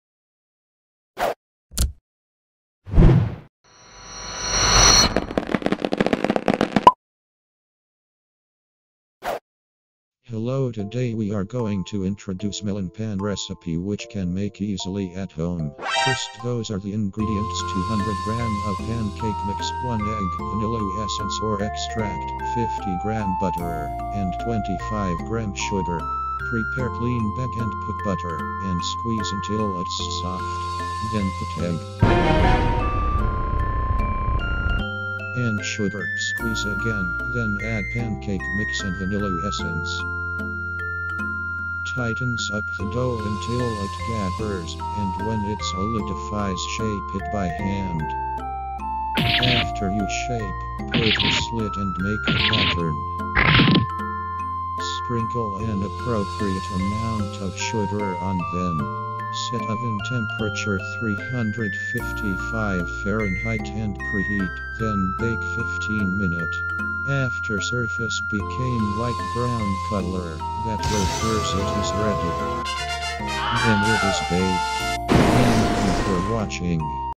Melon Pan Recipe
Melon Pan is a sweet ban invented in Japan.
This video is for to create similar taste sweets at home easily.